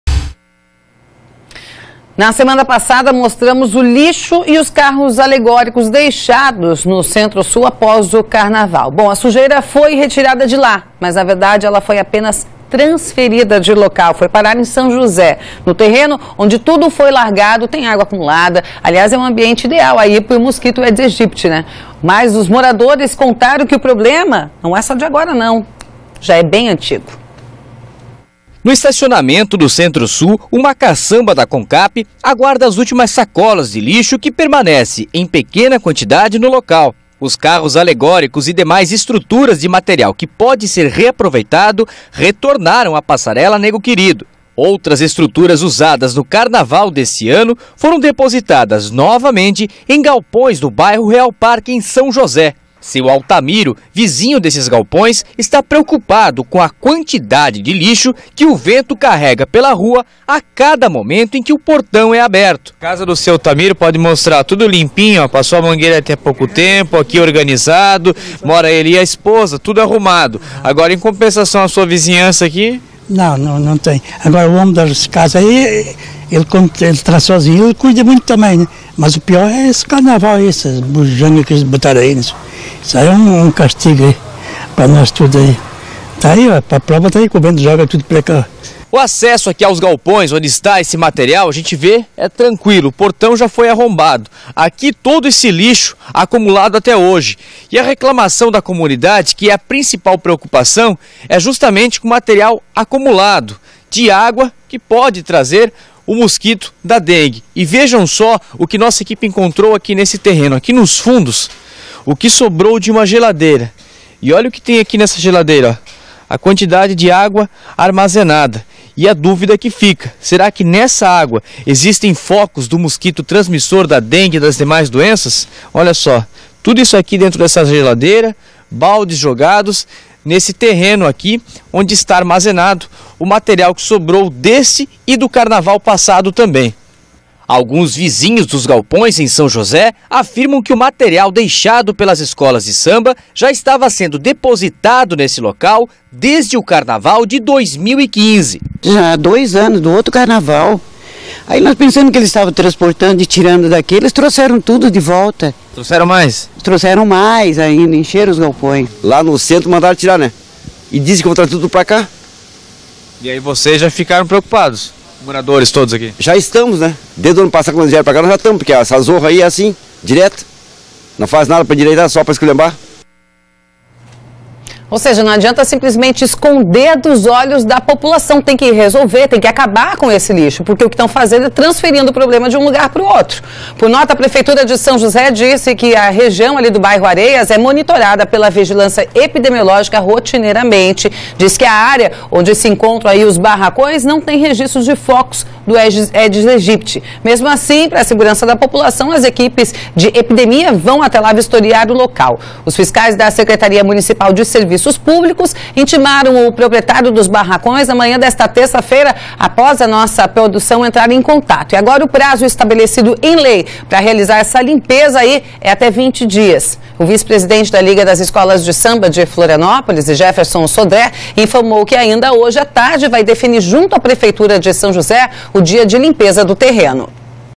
Lixo do desfile de carnaval de Florianópolis foi parar em São José
O lixo deixado pelas escolas de samba da Grande Florianópolis no CentroSul, na Capital, mudou de endereço. Ao invés de dar a destinação apropriada, o que sobrou de alegorias, adereços e carros foi largado em São José.